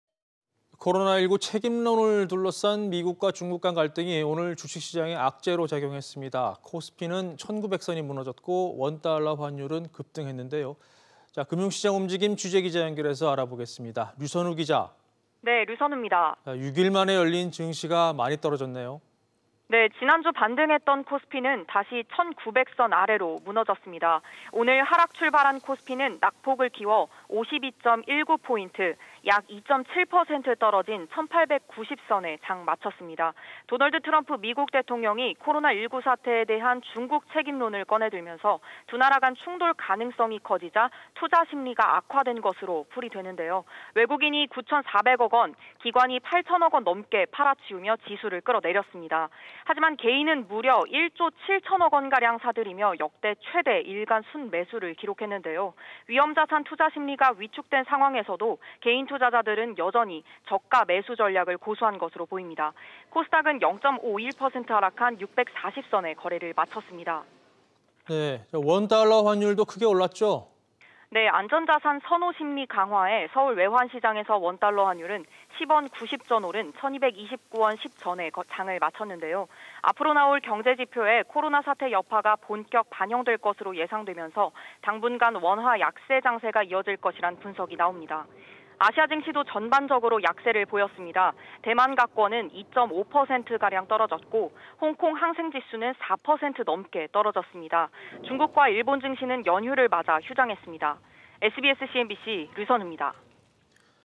미중 무역갈등 재연 우려에…코스피 다시 1900선 ‘붕괴’
[앵커]

코로나19 책임론을 둘러싼 미국과 중국 간 갈등은 오늘(4일) 주식시장에 악재로 작용했습니다.

코스피는 1900선이 무너졌고, 원달러 환율은 급등했는데요.

금융시장 움직임, 류선우 기자 연결해 알아보겠습니다.

6일 만에 열린 증시가 많이 떨어졌네요?